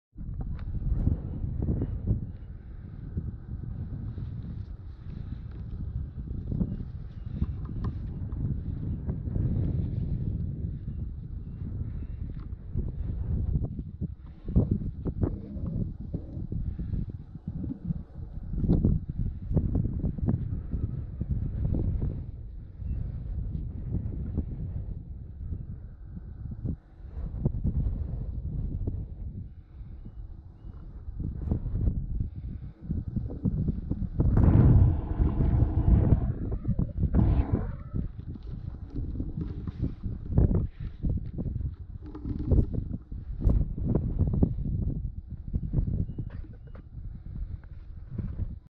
Isle of Skye - The night I spent at Old Man Storr
Isle of Skye - The night I spent at Old Man Storr... I took this video before I went to sleep... The evening was warm... but it had turned really cold when I woke up at 3 am...